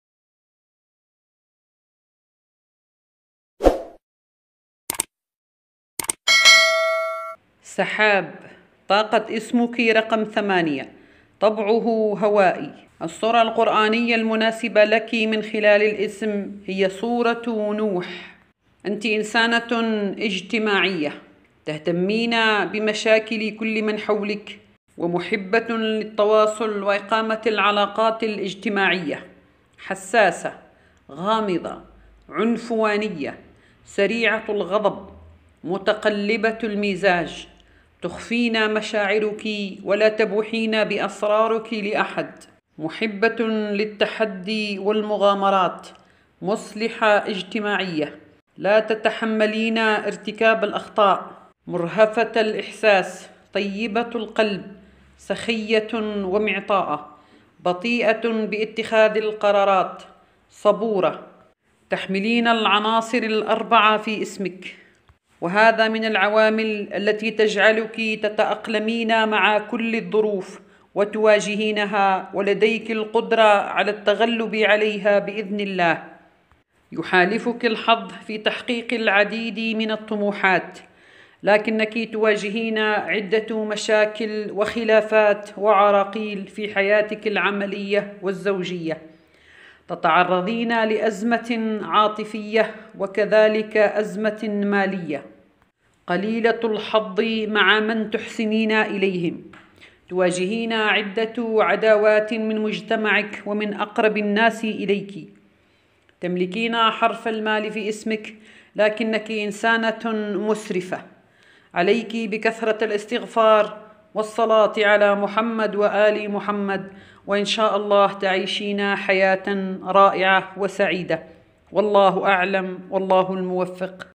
تحليل إسم # سحاب
فيديو يدرس ويحلل إسم سحاب